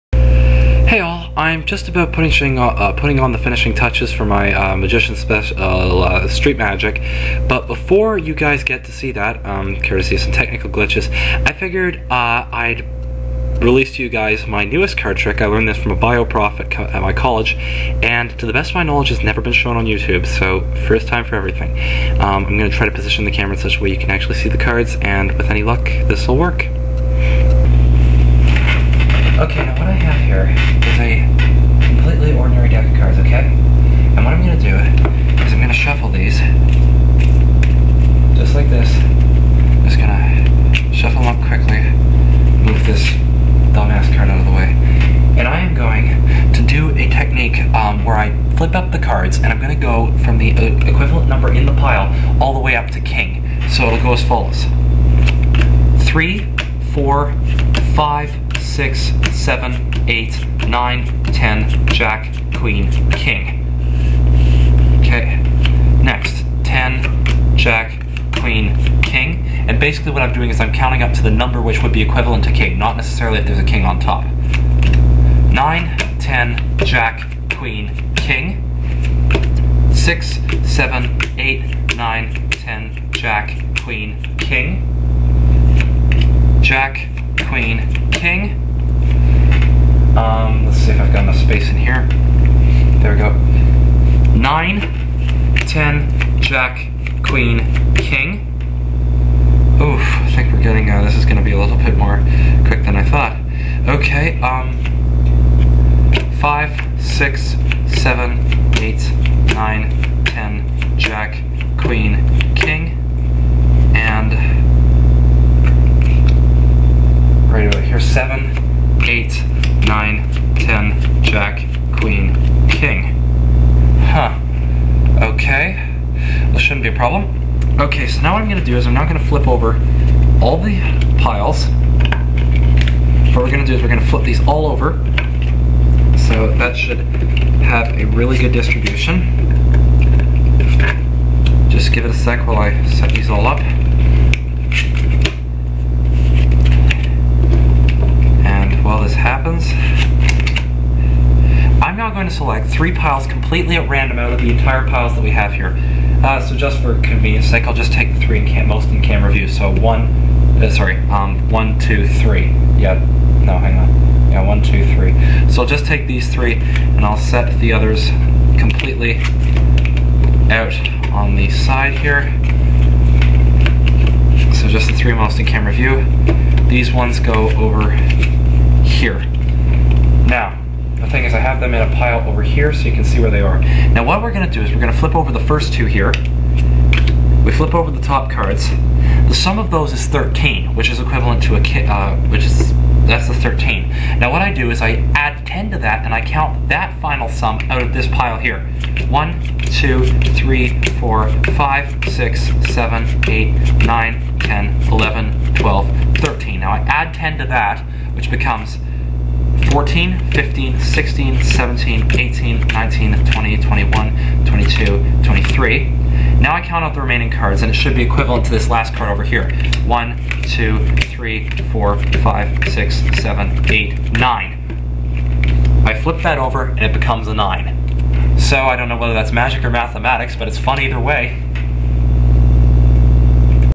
Totally New Card Trick
New card trick, never before shown on youtube. I'm open to ideas as to what to call it.